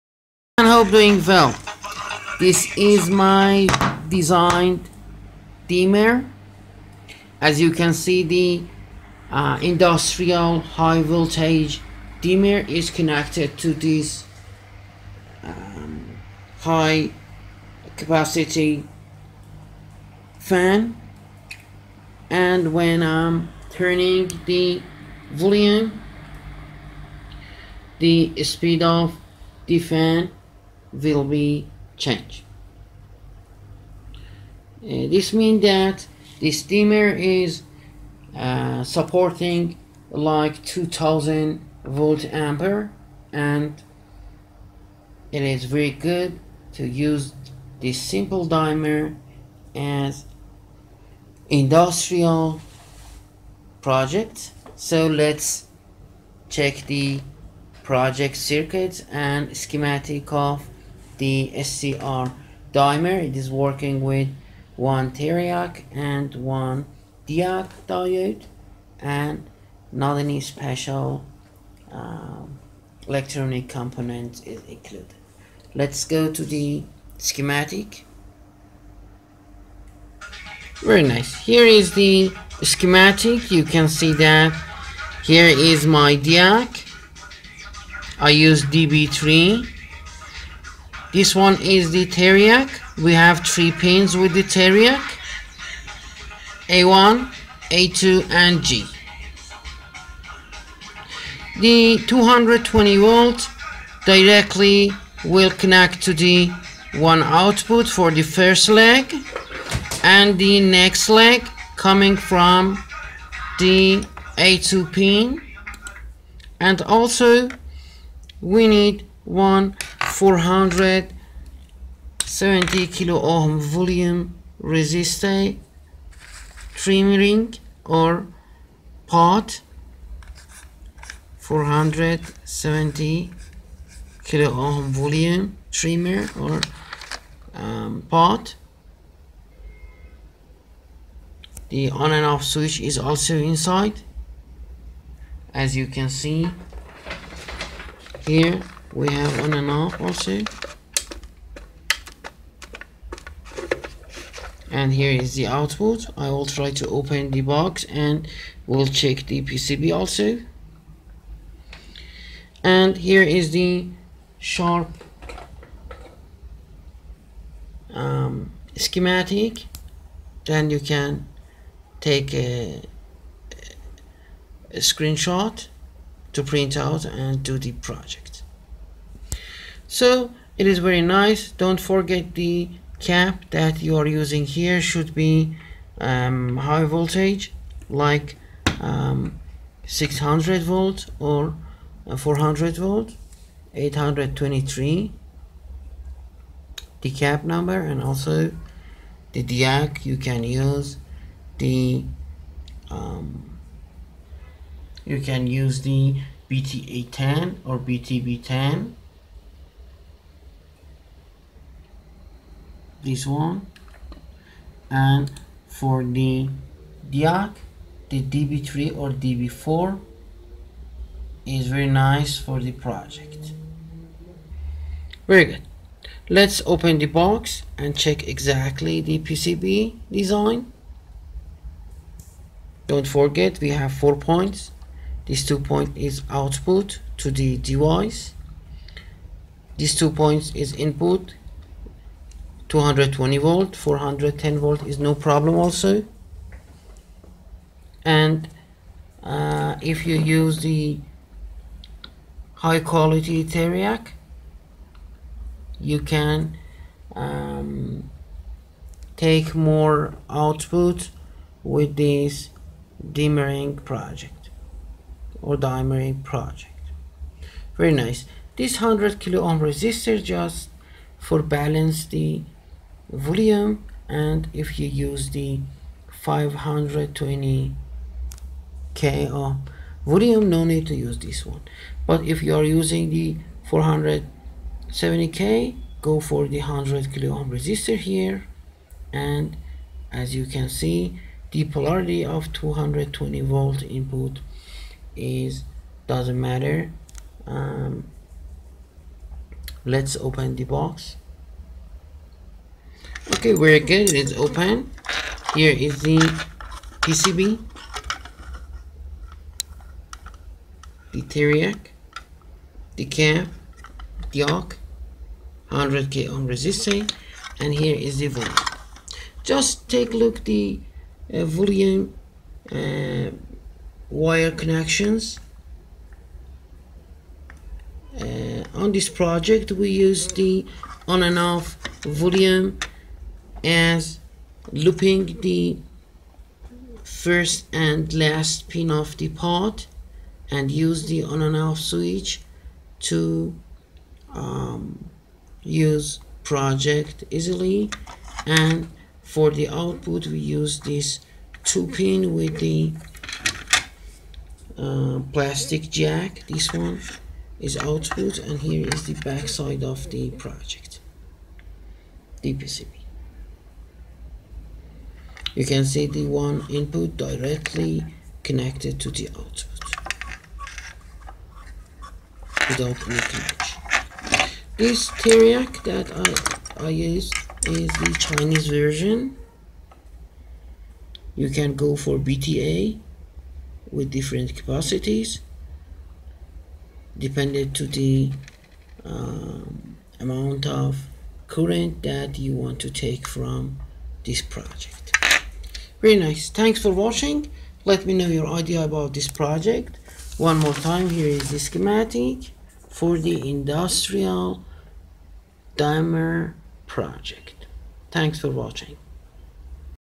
Industrial High Voltage Dimmer (DIY)
on this project, you will see how big cooling fan speed control with industrial easy dimmer and after that we check schematic to well know how the Triac and diac will make nice Dimmer, then we goto open box and check homemade PCB and connection...you can grow up this project when use BTA40 or more to use High Current or high voltage Devices, Thanks for WATCHING.